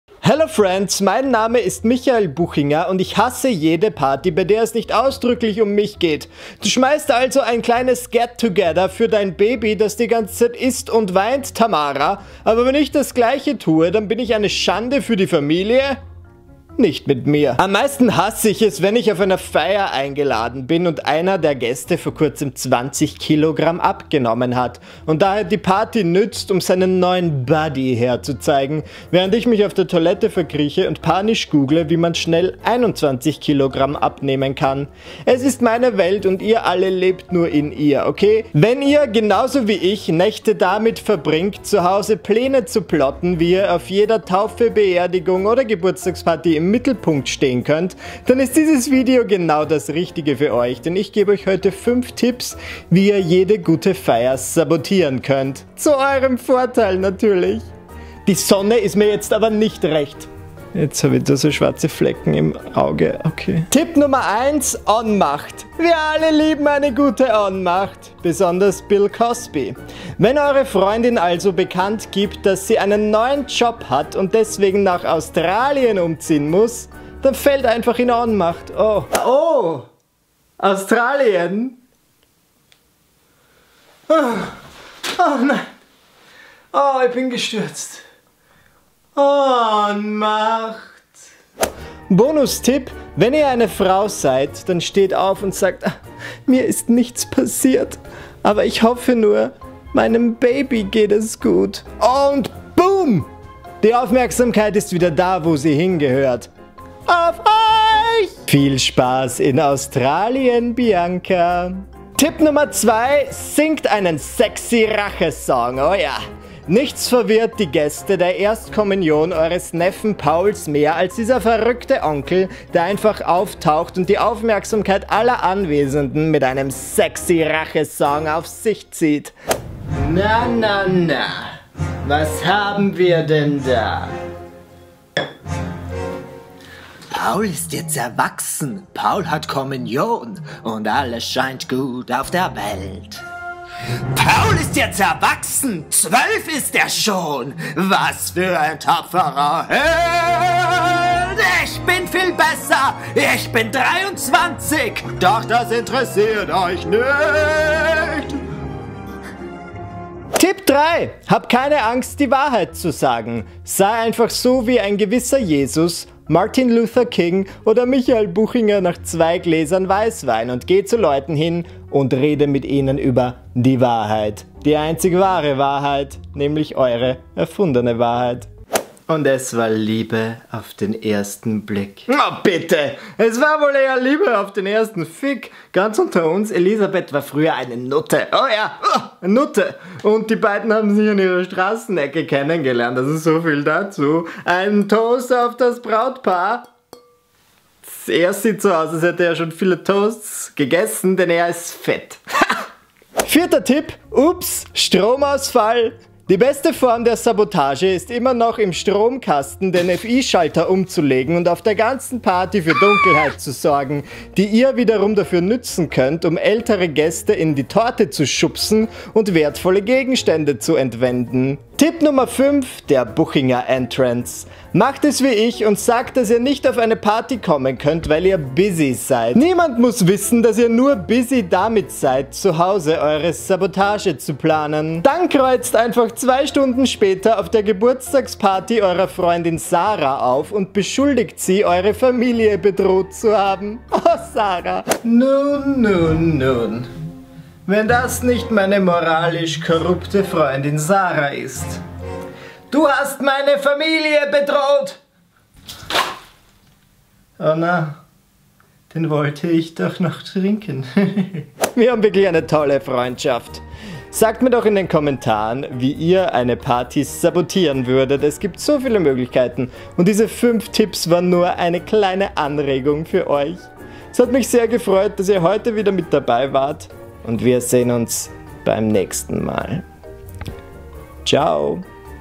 Wie man eine Party sabotiert | Sabotage leicht gemacht!
Wenn ihr so wie Michael Buchinger seid, dann liebt ihr es, auf Partys im Mittelpunkt zu stehen. Damit euch das gelingt, gibt es hier fünf tolle Tipps für euch.

Auf meinem Kanal erwarten euch jeden Freitag merkwürdige Episoden aus meinem Alltag.